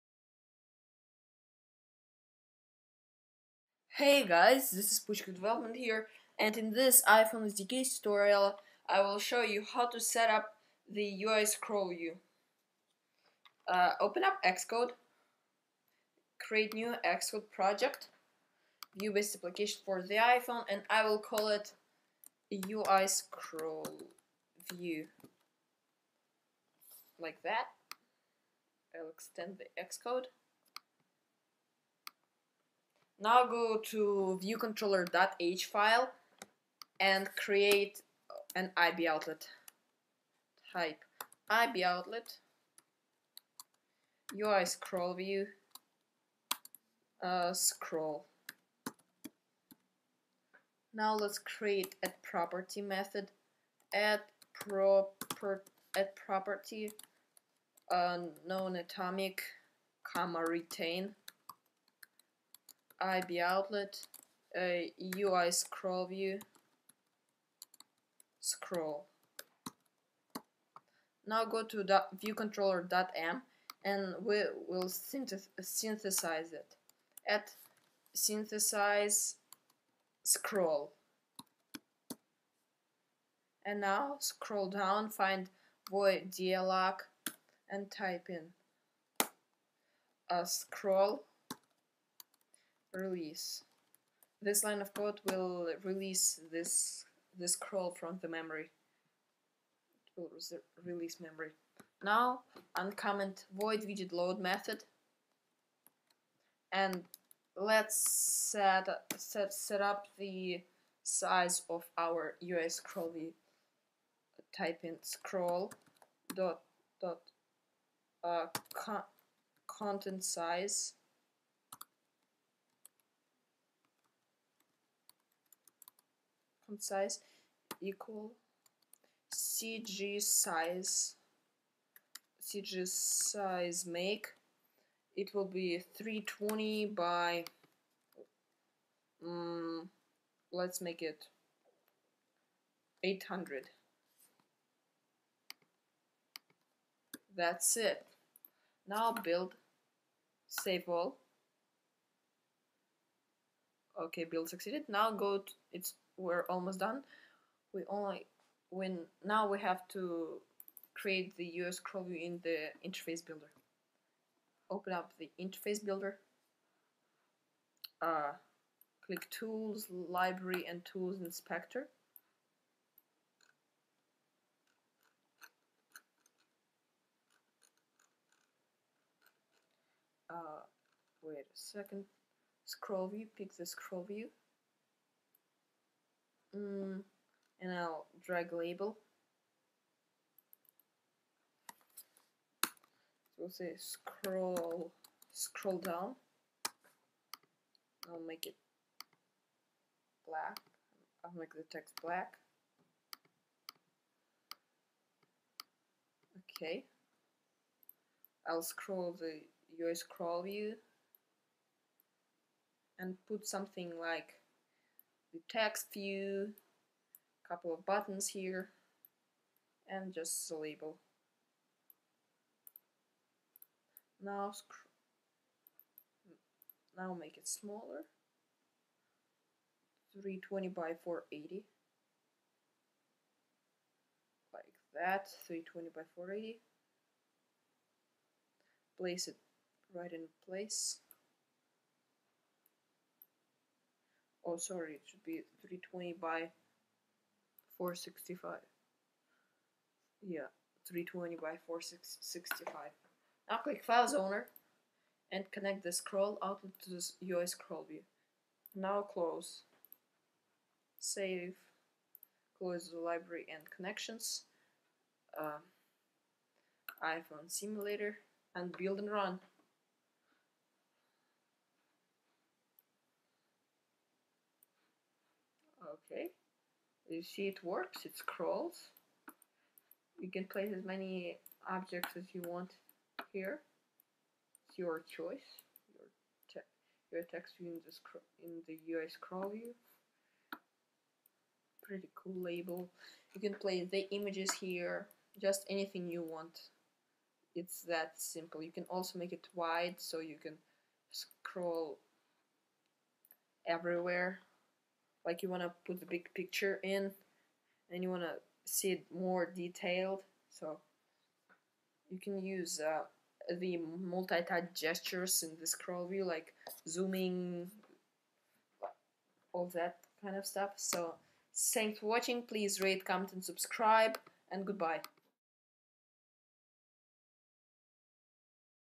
iPhone SDK Tutorial: Using UIScrollView
In this iPhone SDK Tutorial I will show you how to use UIScrollView.